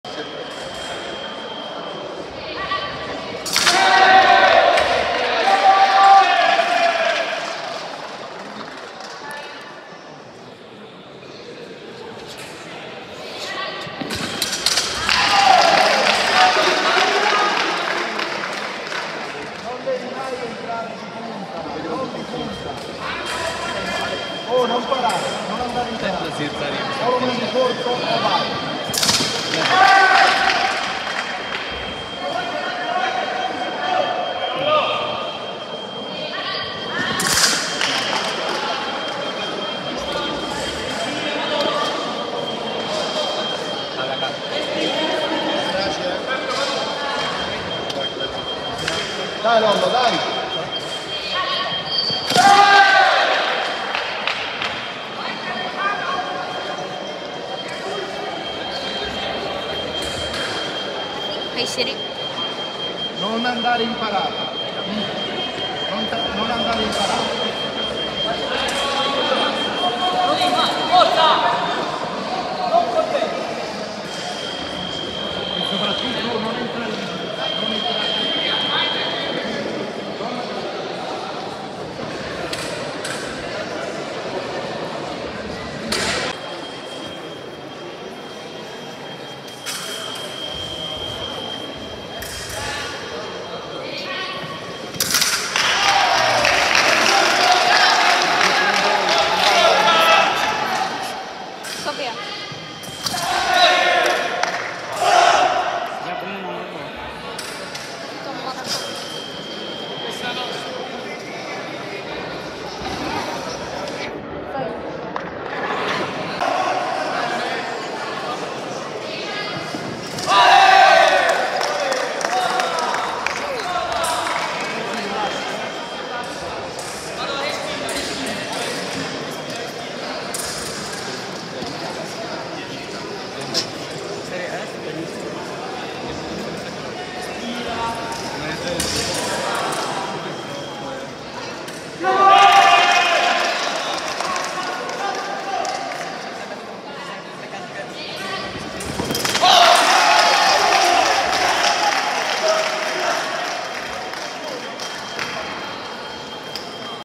ECC Prague 2024 CMS - L8 - Julian Maklakov GER v Lorenzo Madonna ITA
Julian Maklakov from Germany on left side and Lorenzo Madonna from Italy on right side.
Referee: Natalia Nowinowska from Poland
Strip: 3